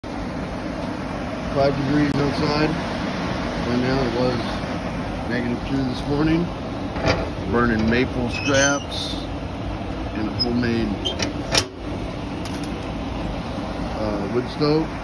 Wood stove blower going now
Homemade wood stove burring trash soft maple wood.  Someone dropped off 4 trailer loads of "free" soft maple logs for the sawmill.  They will make some nice slabs or nice lumber, however with each log also came some of the limb wood.  All of the limb wood, that will fit in the wood stove gets cut up and brought to the shop.   I can heat the shop all winter long with just scrap wood from the Woodmizer sawmill.  The fan on the stove is a nice addition , but a wood burning stove in your workshop is a must.